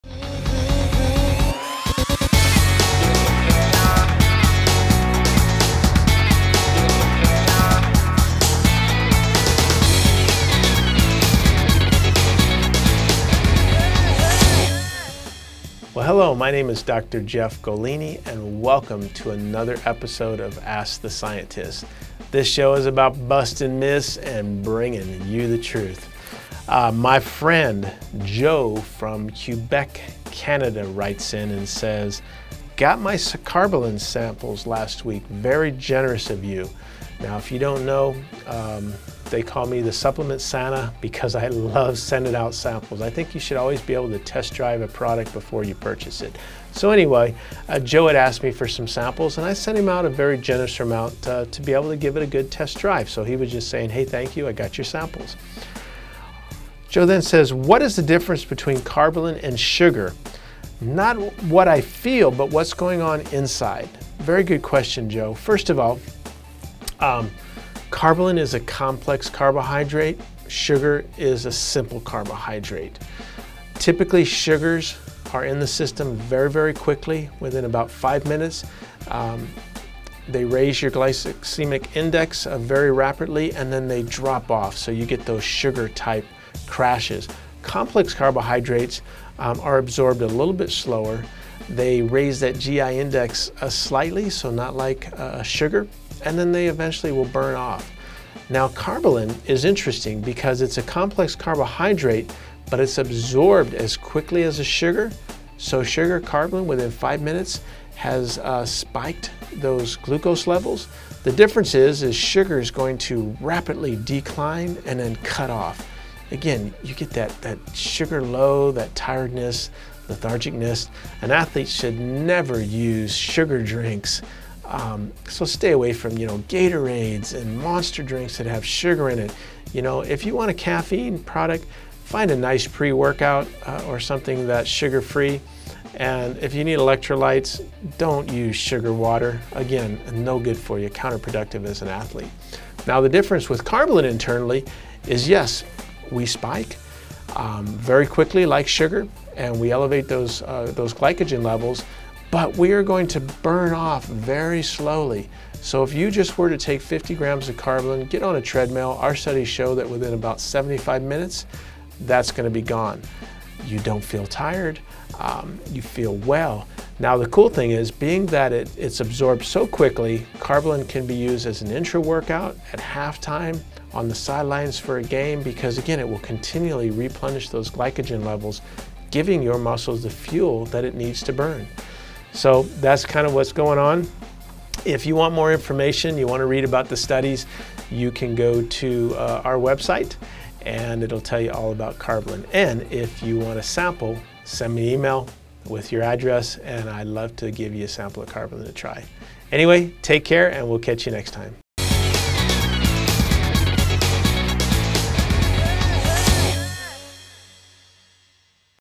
Ask the Scientist - Clarifying the major differences between Karbolyn and sugar
Ask the Scientist - Episode 31

In this episode, Dr. Jeff Golini clarifies what the major differences are between Karbolyn and sugar, and how each affects the body.